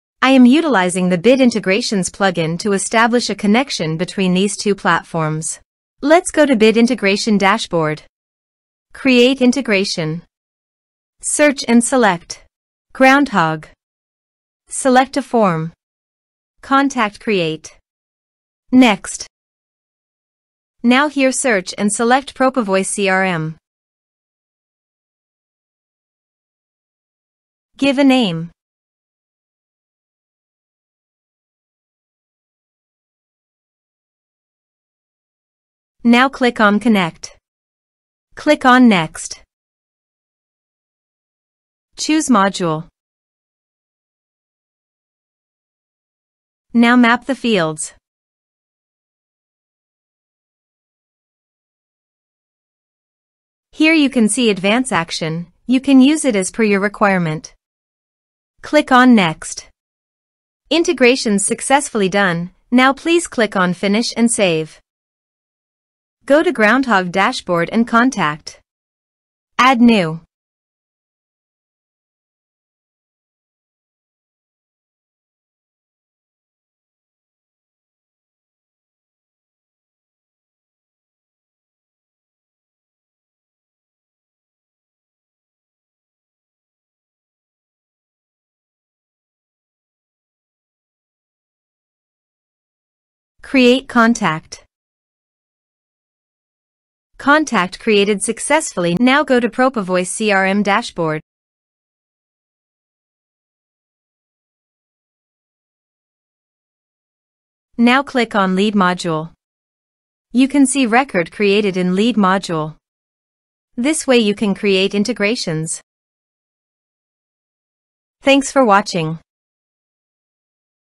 (Free) Integrating Groundhogg with Propovoice CRM | Step-by-Step Tutorial | Bit Integrations
🔗 Ready to optimize your customer relationship management (CRM)? Join us on our YouTube channel for an in-depth tutorial where we reveal the seamless integration of Groundhogg with Propovoice CRM, a comprehensive CRM solution designed for businesses. Learn how to effortlessly connect these two tools using the Bit Integrations WordPress plugin.

Follow along as we guide you through the step-by-step process of integrating Groundhogg with Propovoice CRM, empowering you to streamline lead management, client communication, and business automation. Say goodbye to manual data transfers and welcome automated workflows for heightened efficiency in managing your customer relationships.

Key Highlights:
✅ In-depth walkthrough for integrating Groundhogg with Propovoice CRM using Bit Integrations for WordPress.
✅ Leverage your form data to streamline lead management, client communication, and business automation with Propovoice CRM.
✅ Elevate your CRM strategy with streamlined automation and enhanced productivity.

Ready to revolutionize your CRM experience?

Unlock advanced CRM capabilities:
📧 Elevate your CRM endeavors with the Pro version of Bit Integrations

Whether you're a business owner, sales professional, or CRM enthusiast, this integration empowers you to seamlessly merge Groundhogg with Propovoice CRM. Watch now to revolutionize your CRM efforts!

🎥 Ready to revolutionize your CRM strategy? Join us in optimizing your customer relationships with seamless integration! 👍